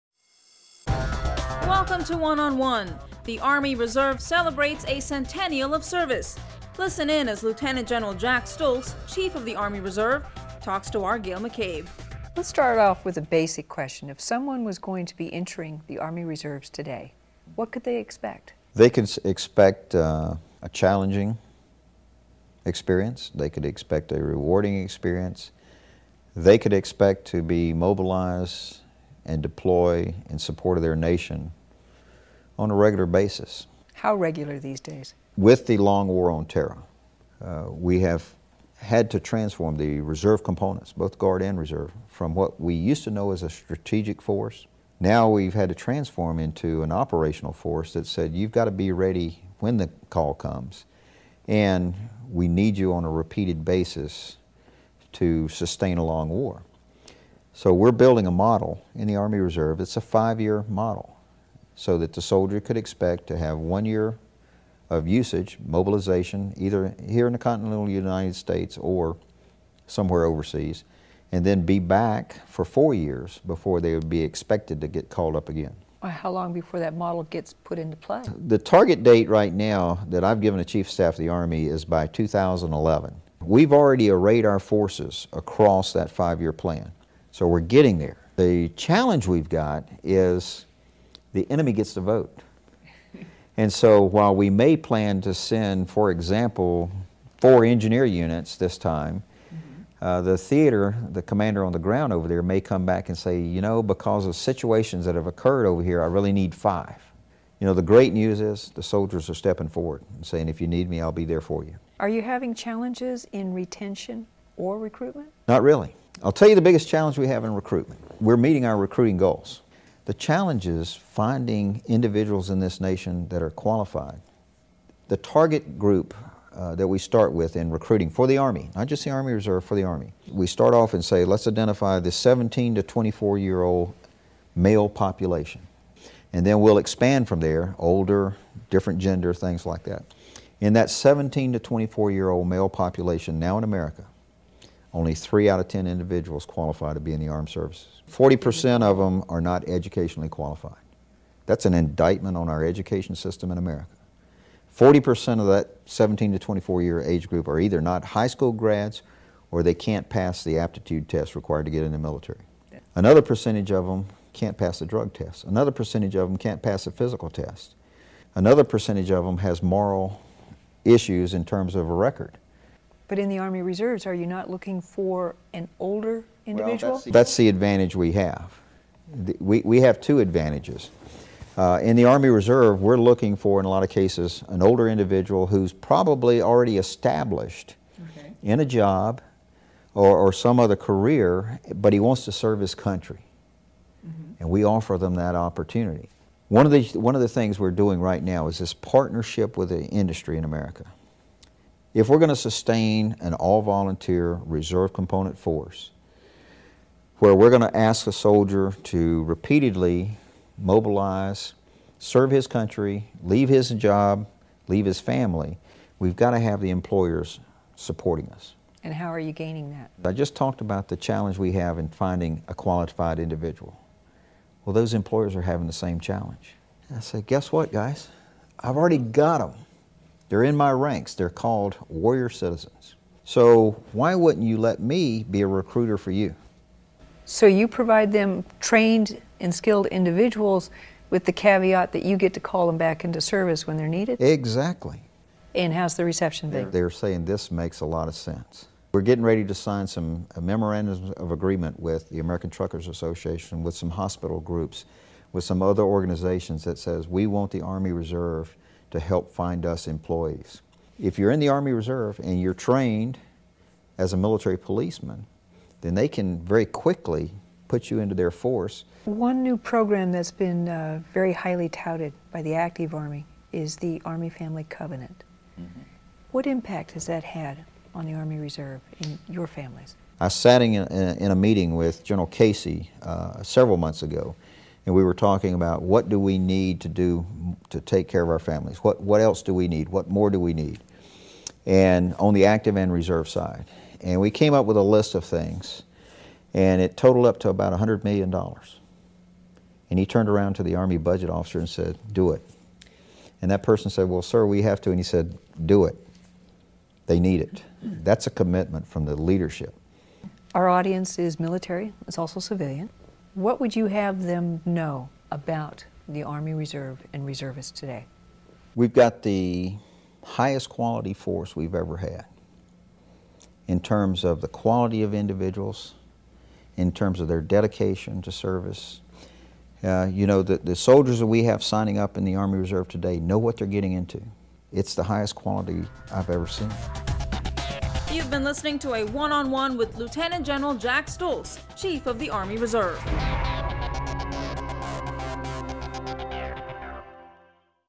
One on One - Lt. Gen. Jack Stultz
The Army Reserve celebrates a centennial of service. Listen in as Lt. Gen. Jack Stultz, Chief of the Army Reserve, speaks with SRTV's Gail McCabe.